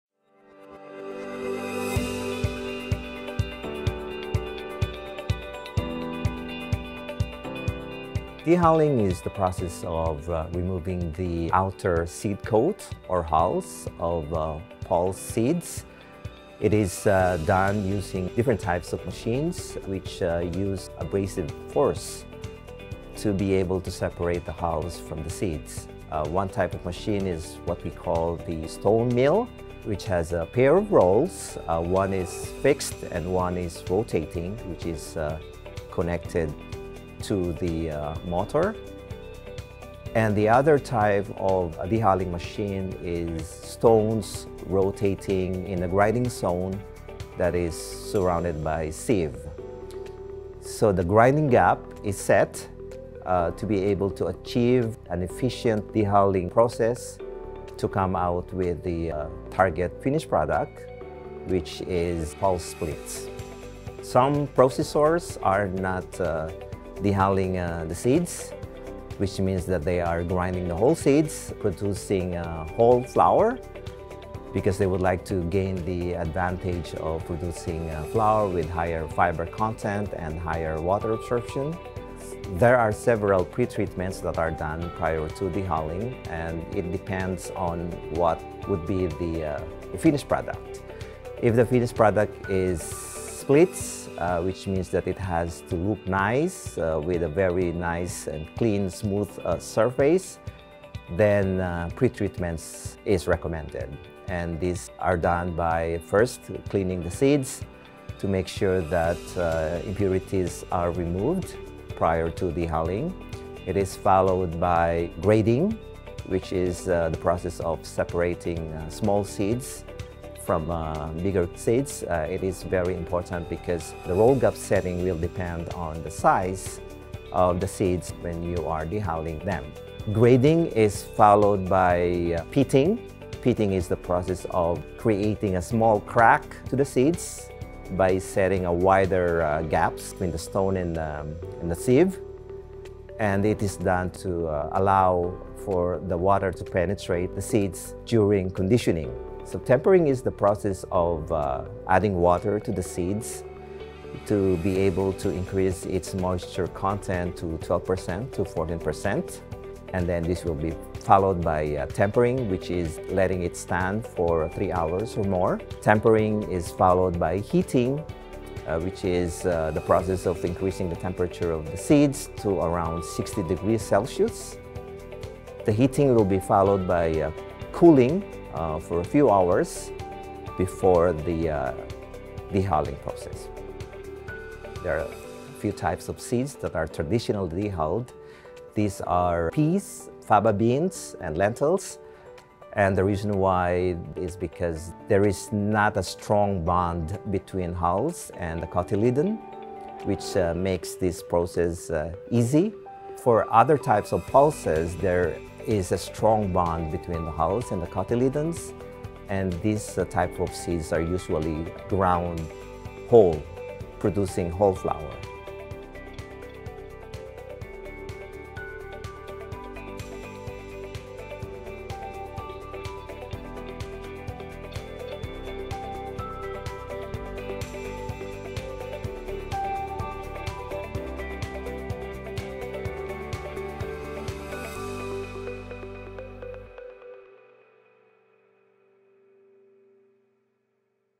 Pulse Flours 101: The Dehulling Process
Pre-treatments, milling method and milling settings all have a big impact on the final appearance, taste and functionality of pulse flours. Join Norbert Cabral of Cereals Canada to learn about a common first step in producing pulse flours - dehulling.